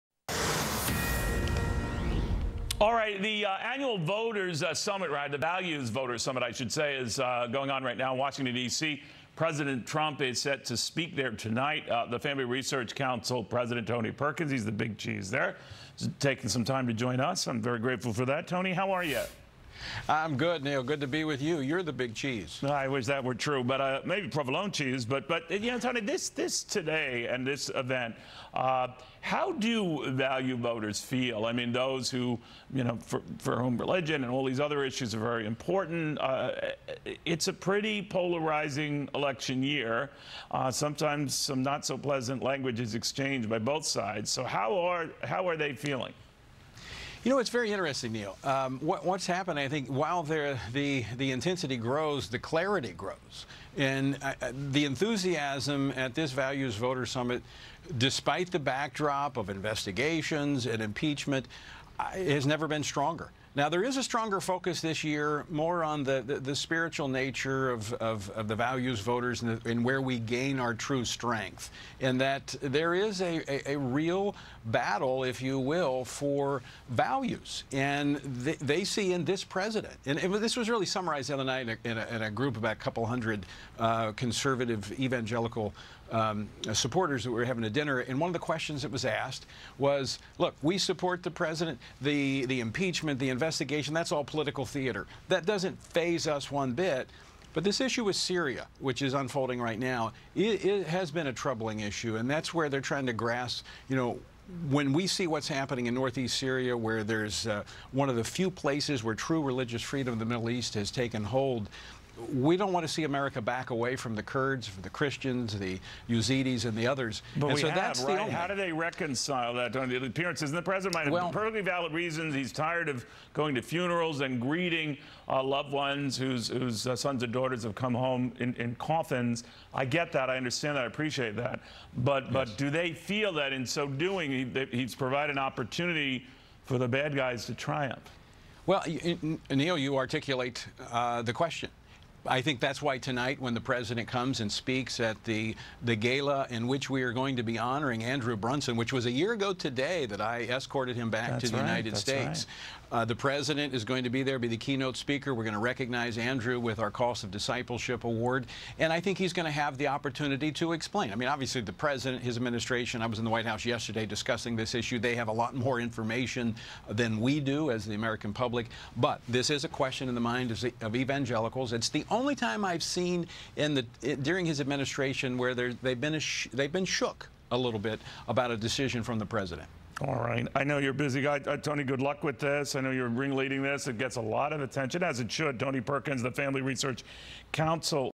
Tony Perkins Talks the Values Voter Summit
Tony Perkins discusses the focus of the Values Voter Summit and what values voters think of the current political climate.